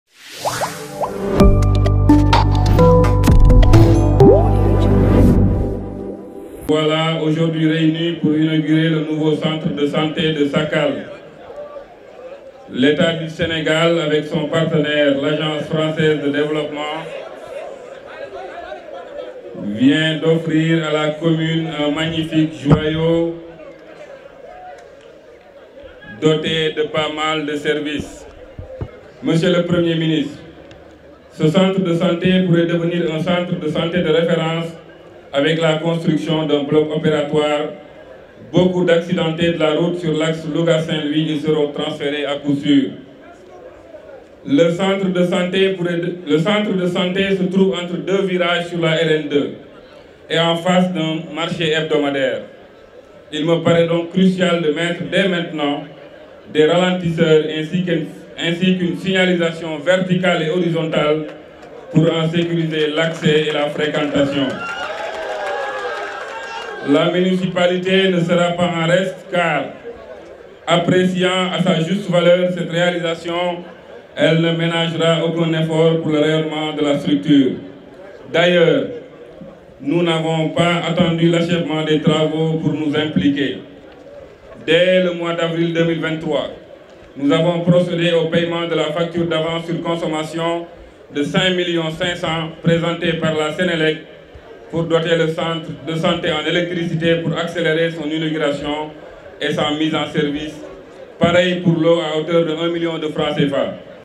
Louga : Inauguration du centre de santé de Sakal
La municipalité a alloué 6,5 millions de francs CFA pour l'électricité et 1 million de francs CFA pour l'eau afin de faciliter l'inauguration et la mise en service du centre de santé de Sakal.

Ousmane Sakal Dieng, le Maire de la commune de Sakal, a exprimé sa gratitude au Président Macky Sall et à son gouvernement au nom de toute la population de la commune de Sakal, comprenant les 133 villages et 25 hameaux, ainsi que les régions avoisinantes. Il a qualifié le centre de santé de Sakal de « bijou » en soulignant qu'auparavant, les habitants étaient contraints de se rendre à Louga ou Saint-Louis pour bénéficier de soins pour de nombreuses pathologies.

Selon le Maire, ce centre de santé constitue un élément crucial dans l'amélioration du bien-être des bénéficiaires, marquant ainsi une avancée significative dans la prestation de services de santé à la population.